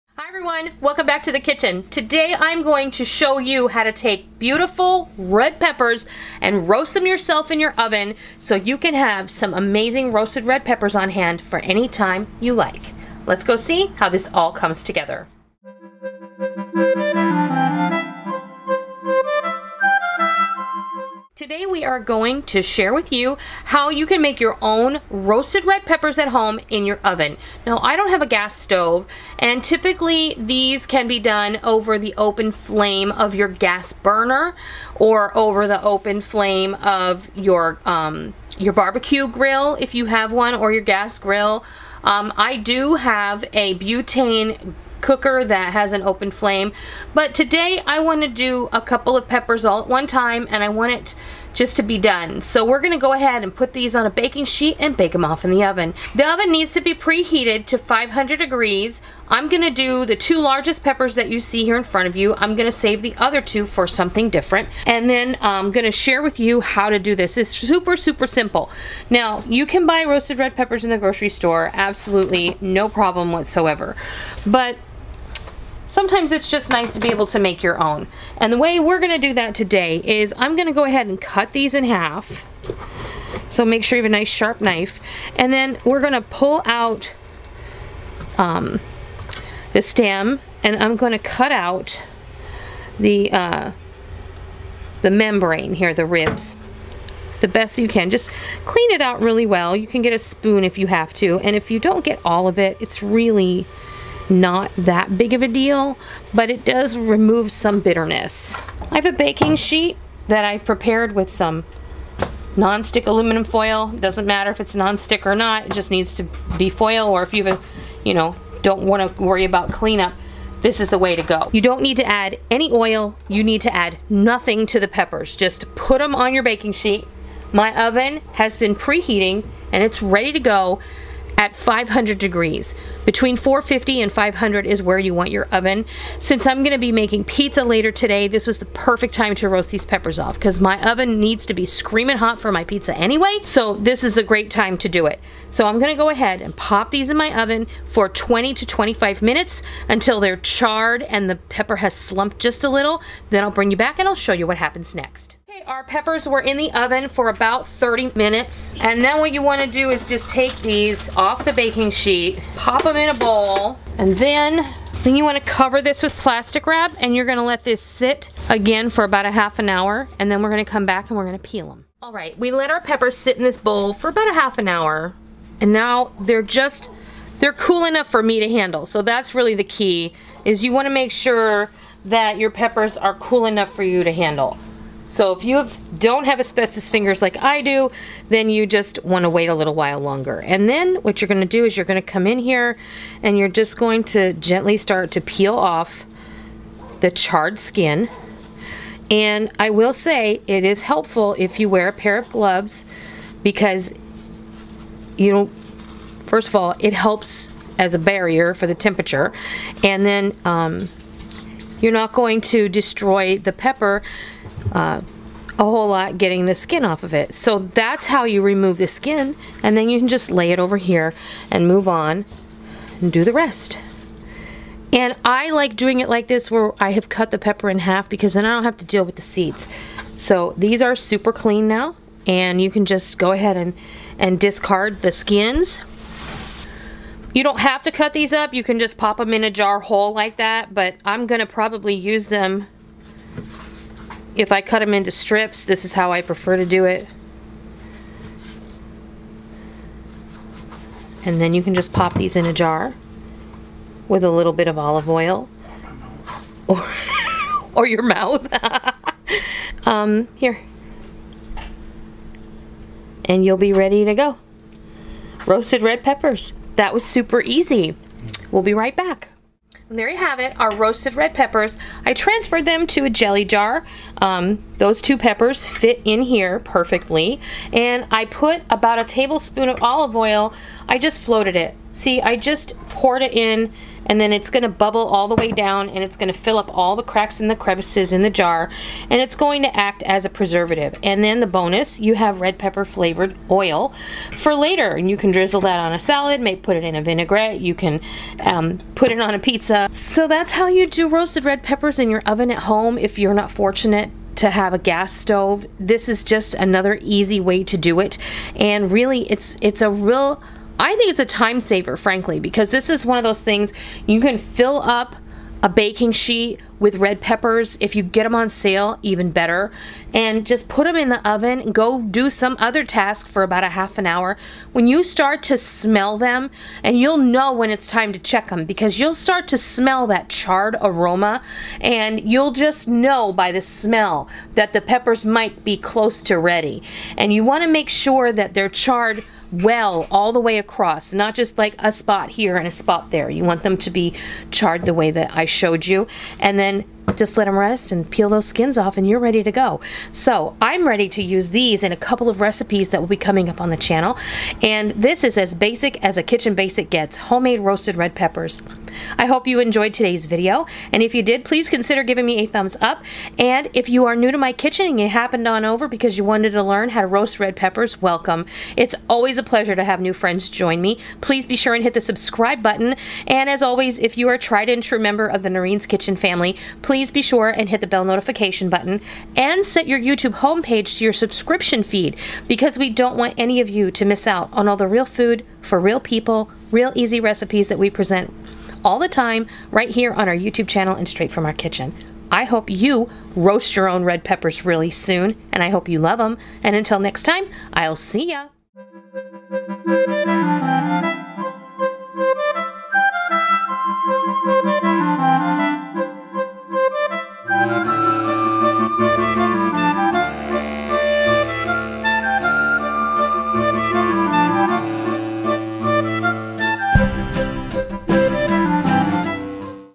HOW TO OVEN ROAST PEPPERS
NOTE:  This recipe is not appropriate for canning
PLEASE READ INFORMATION BOX REGARDING STORAGE

Today I am sharing a real kitchen basic!  How to oven roast red peppers.  These are delicious additions to salads, pizzas, and even the ever popular pimento cheese that we love.  Making your own ensures consistent quality and if you are able to get a sale on red peppers this is a great way to preserve them for later use.  Roasting give these a slightly smoky flavor and it also makes the peppers more digestible.  Here are all the tips and tricks you need to know to roast your own peppers at home in your oven.

I roast my peppers in the oven because I do not have a gas stove where I can roast them over an open flame.  You can do it that way or on your grill over the flame, but doing it in the oven helps to do a bunch at one time and get the job done while you multi task.

I have only done two peppers today but you can make as many as will fit on your tray.  I would love to do a big bunch and fill a quart jar to keep in my fridge during the holiday season.  They are great additions to other recipes but they are delicious to add to an antipasto platter or to a nosh spread with cheeses crackers and other olives and crudite.

This process is super simple. Preheat your oven to 500 degrees. I like to wash and dry my peppers.  then cover a baking sheet with foil.  Cut your peppers in half and remove the seeds and the ribs.  This can be done with a spoon for easy removal.  Lay the peppers cut side down on the baking sheet.  Pop in the oven for 25 to 30 minutes or until the pepper skin is completely charred.  You will be able to smell the peppers as they roast and will know when to check them.

Once the peppers are done cooking and the skin is well charred, remove from the baking sheet, using tongs and place the now soft peppers into a bowl.  Cover with plastic wrap and allow to rest for 30 minutes or until cool enough to handle.

I like to use rubber gloves to remove the skin.  You will take a pepper half and lay it on a plate or a cutting mat then gently begin to remove the skin which should come off quite easily.  Continue to do this until they have all been done.  I like to pile my peeled peppers on the corner of the mat and then slice them into strips with a sharp knife.

Place the pepper strips into a mason jar and float a bit of olive oil over them.  This will help to preserve them while they are stored in the refrigerator.  These will last up to three months in the refrigerator with the olive oil  If you store them without the oil they will last up to two weeks.

I hope you give oven roasting red peppers a try sometime soon and I hope you love them!

Happy Eating!

Knife I used is the Rada Cooks Knife and my favorite knife in the world!

***MAILING ADDRESS
Noreen's Kitchen